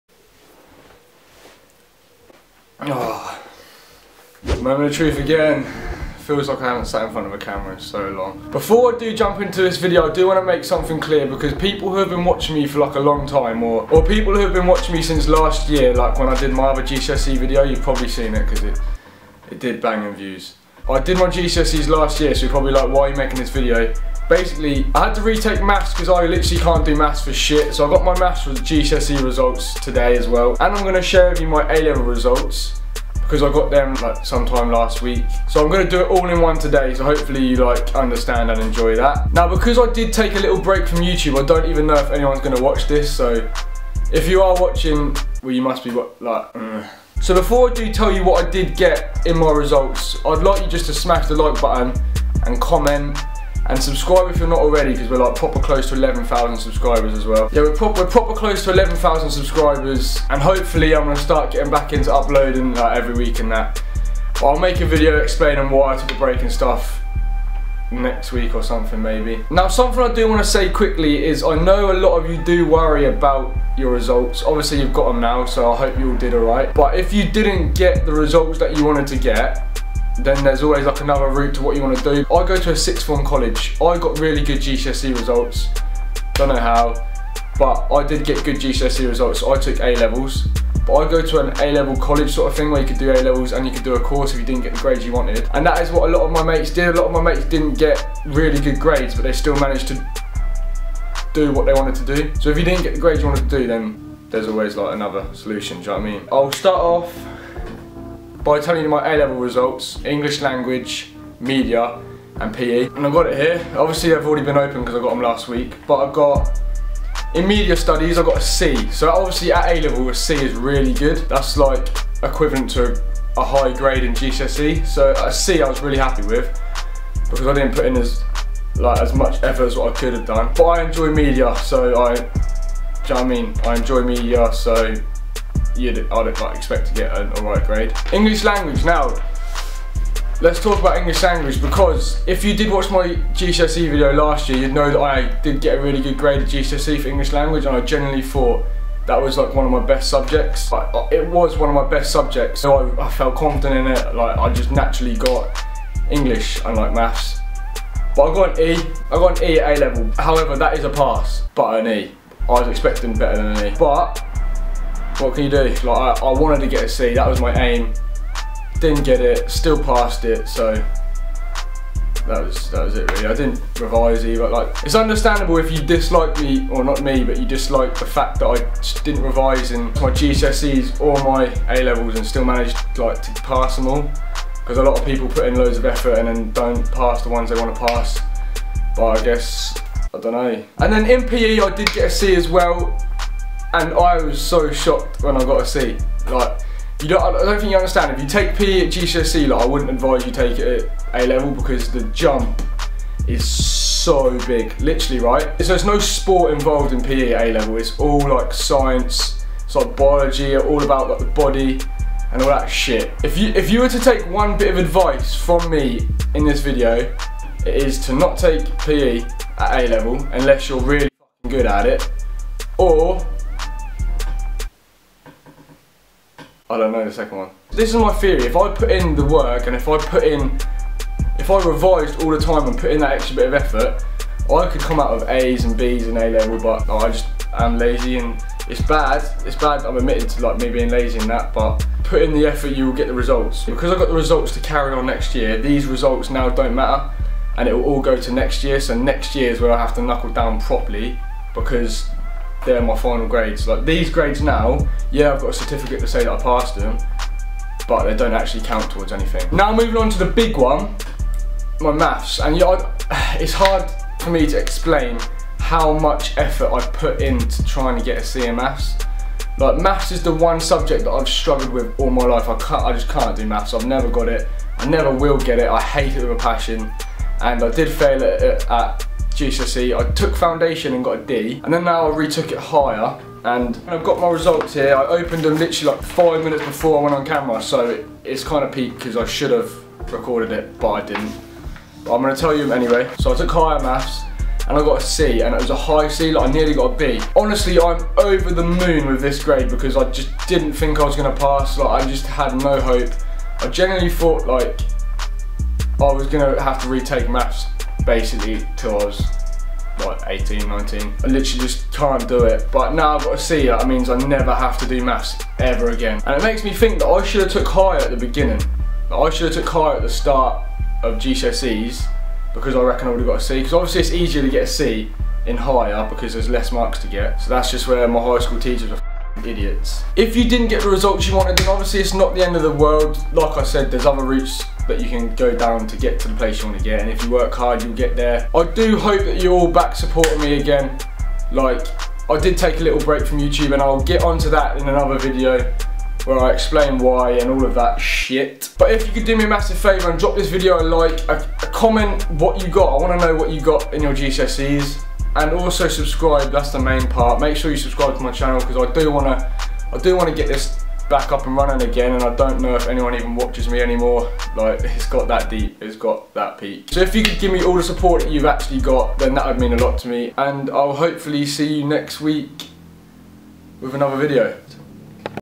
OPENING MY GCSE & A-LEVEL RESULTS 2017 (REACTION)
I'm back! Hope youre all doing gooood, heres me reacting to my GCSE and A-LEVEL results 2017. I need all the support i can get at the moment as i have no clue if anyone even watches me anymore lol.